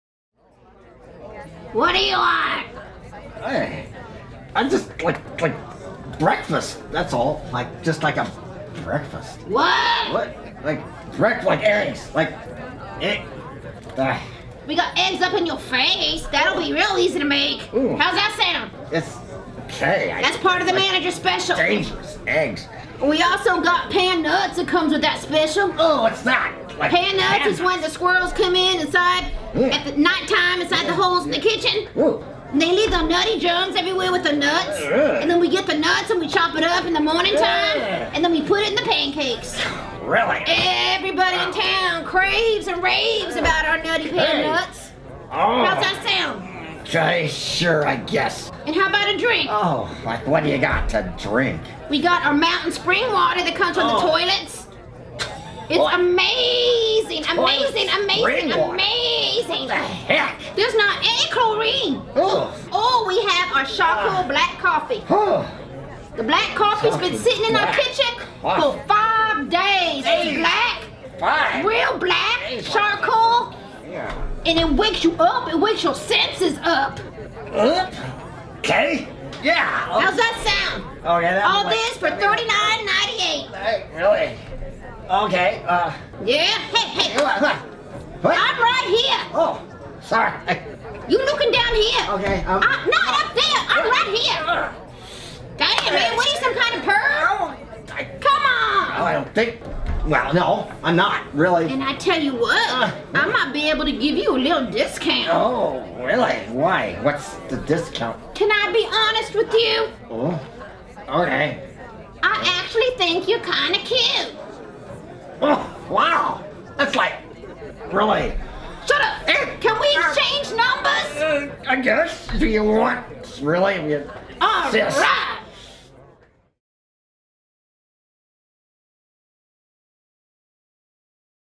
'' At The Diner '' -- Ad-Lib Comedic Scene --- Larry Hodges - Monique Barbell
Edga [ Larry Hodges ].ordering breakfast from waitress [ trixi ] Monique Barbell, at The Greasy Spoon diner in this ad-lib comedic scene.
Concept and directed by Larry Hodges.